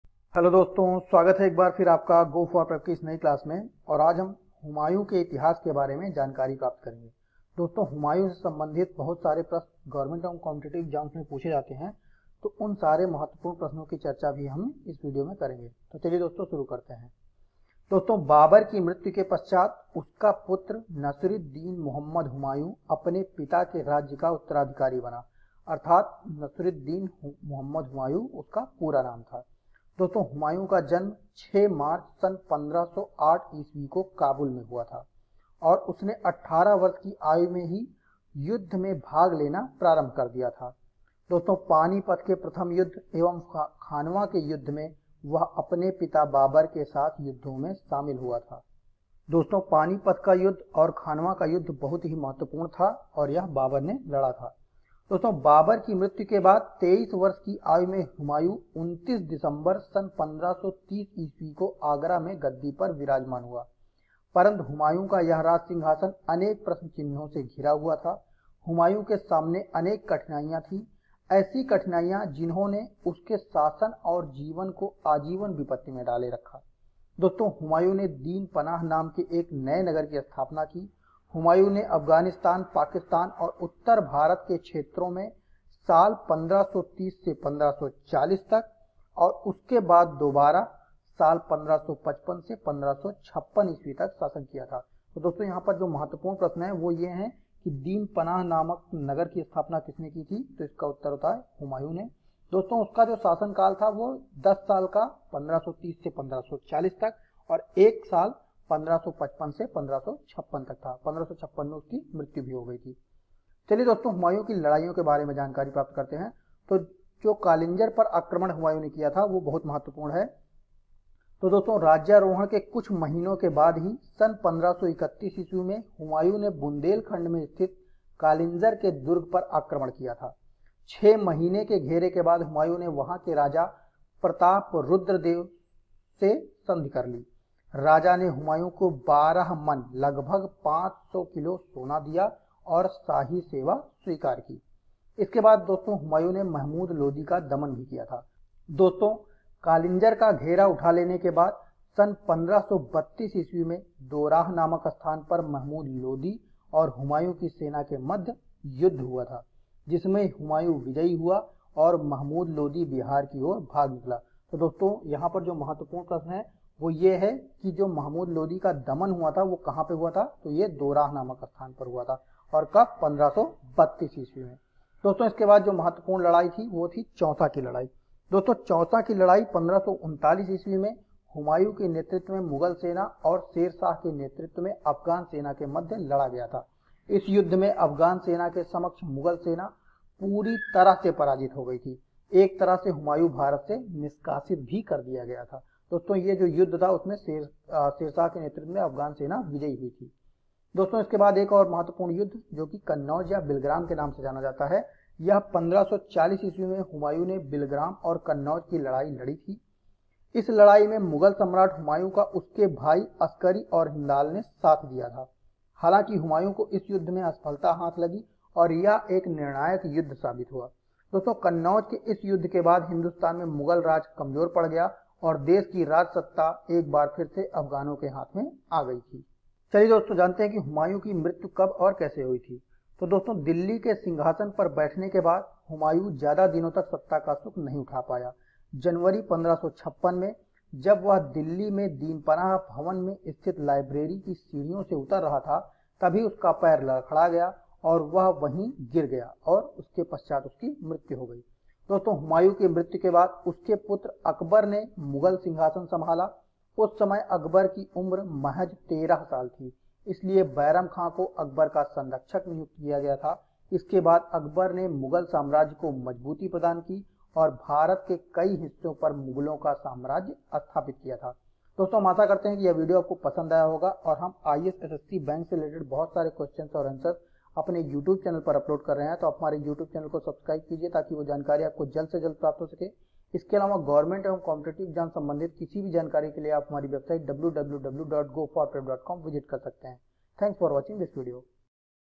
Humayun History In Hindi हुमायूँ का इतिहास
Humayun Ka Itihas & Biography information In Hindi
मुगल बादशाह हुमायूं की जीवनी, जानिए दूसरे के बारे में सभी तथ्य भारत का मुगल शासक के बारे में | हुमायूं एक मुगल शासक था|
Humayun History in Hindi Children & Son, wife Akbar History in Hindi
More History of Humayun In Hindi

00:00 - Intro
00:17 - Humayun History In Hindi हुमायूँ का इतिहास
00:17 - Humayun Story in Hindi
01:55 - कालिंजर पर आक्रमण
02:25 - हुमायूँ और महमूद लोदी
02:55 - हुमायूँ चौसा का युद्ध
03:20 - कन्नौज का युद्ध
03:49 -  हुमायूँ  की मृत्यु कब और कहाँ हुई थी?